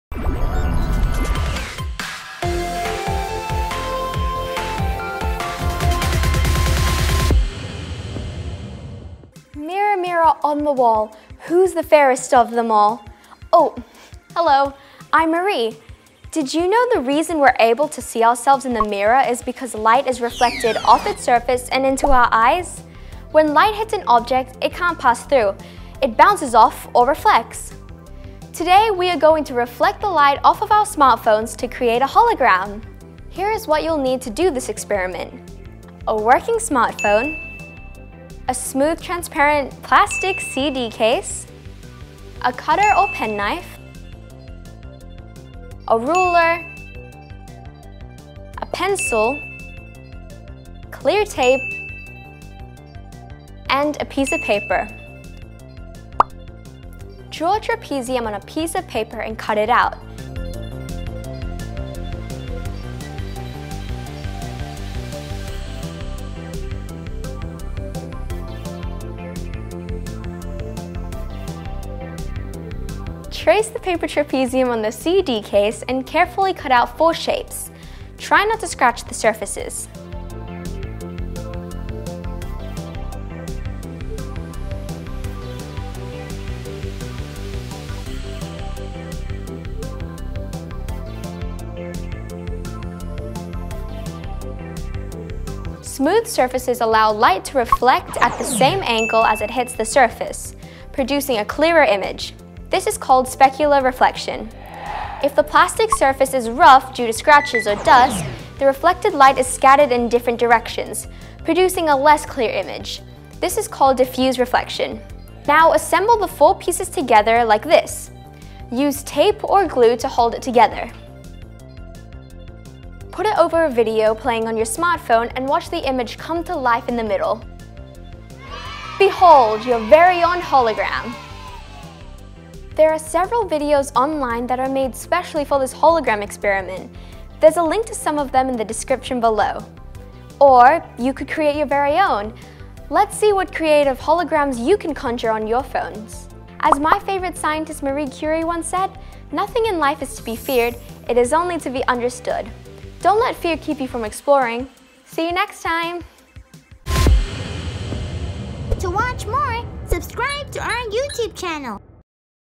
Awesome Light & Holograms Experiments at Home! | Argo's World | STEM for Kids
🌟 Welcome to Argo’s World! 🌟

Hello young scientists! Join Marie in her experiment on how reflected light from our smartphones can be used to create a hologram, which you can carry out in the comfort of your home!

** Remember to seek adult supervision and always wear your safety goggles! **